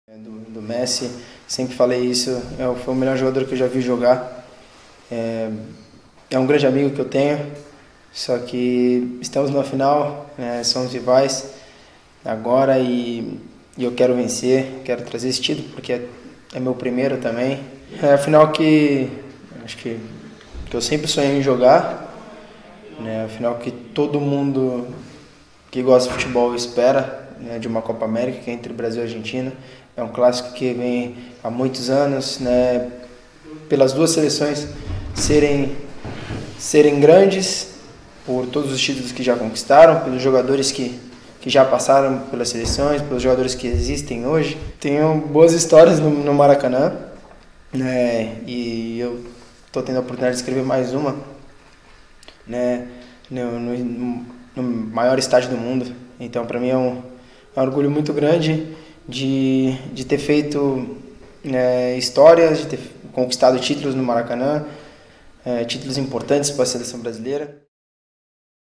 Neymar trifft auf Messi: "Bester Spieler, den ich gesehen habe" | SID
Das Traumfinale bei der Copa América steht unmittelbar bevor. Das Duell der Schwergewichte Brasilien gegen Argentinien ist auch eines der Superstürmer Neymar und Lionel Messi. Der Star der gastgebenden Seleção spricht trotz aller Rivalität in höchsten Tönen vom Ausnahmekönner Argentiniens.